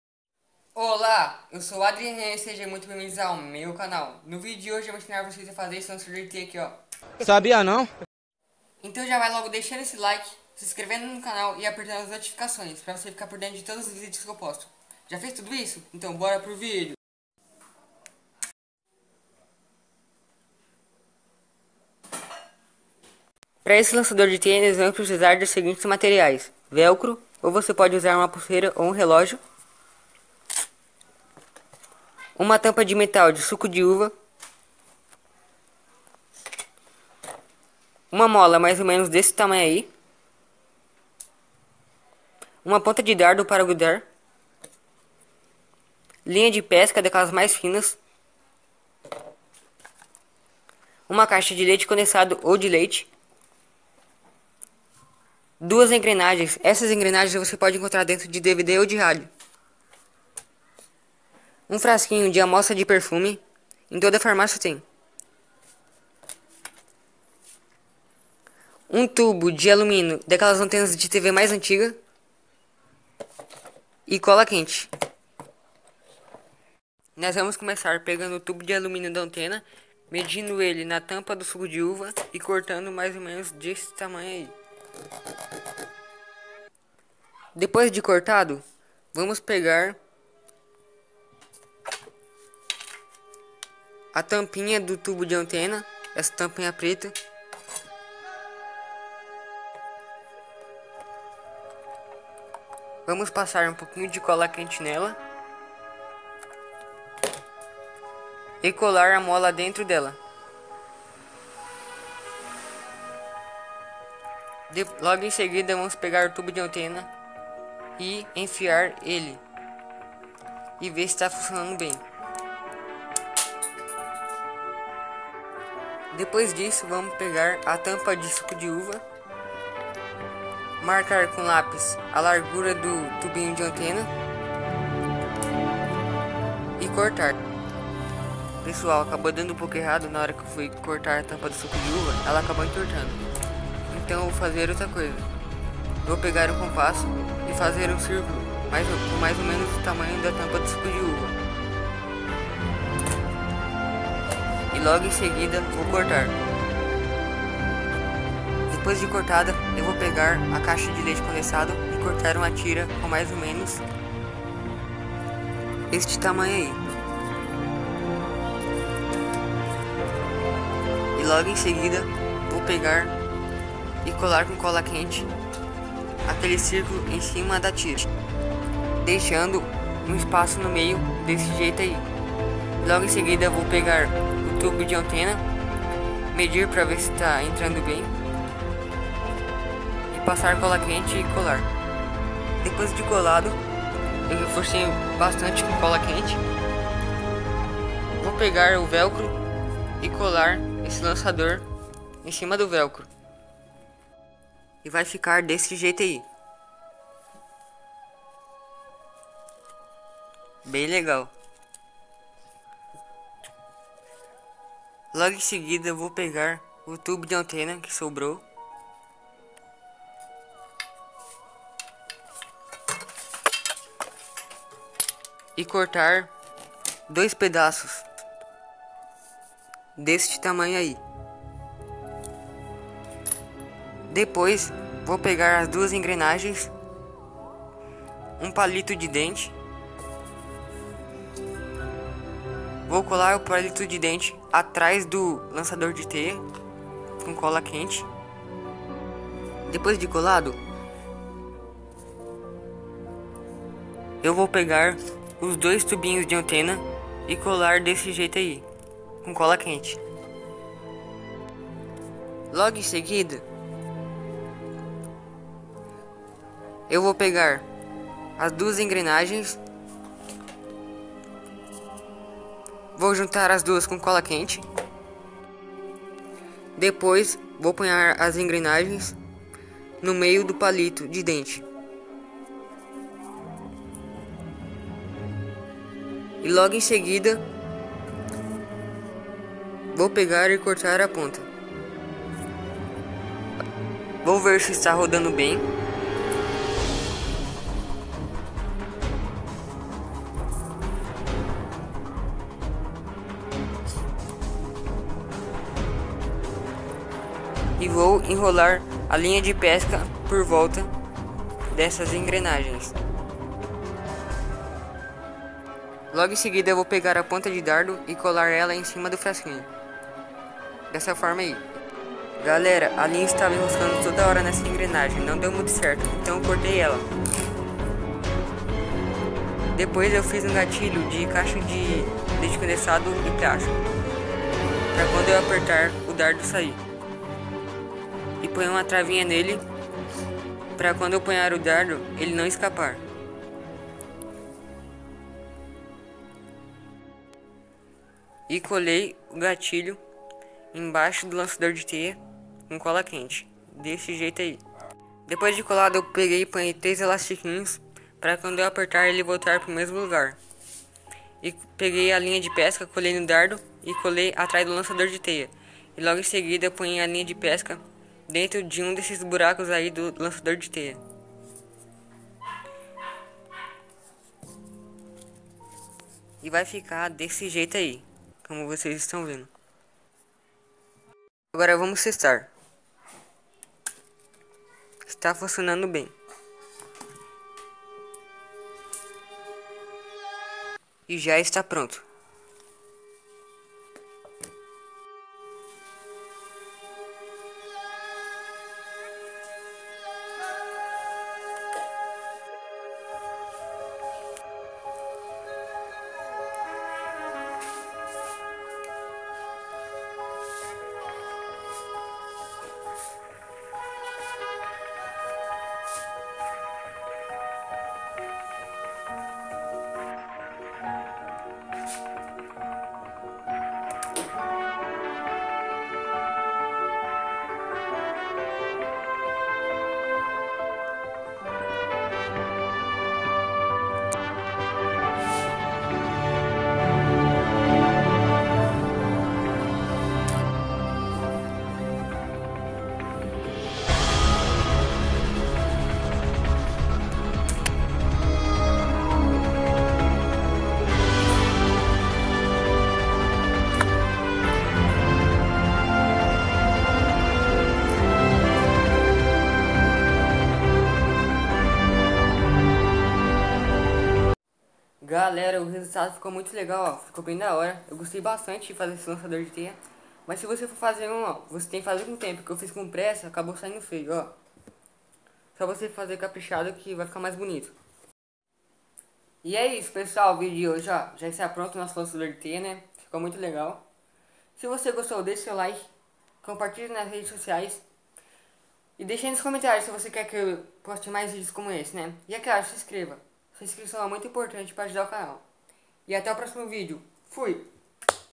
Como fazer um Lançador de Teia do Homem Aranha.
Neste vídeo vou ensinar a como fazer um Lançador de Teia do Homem Aranha caseiro.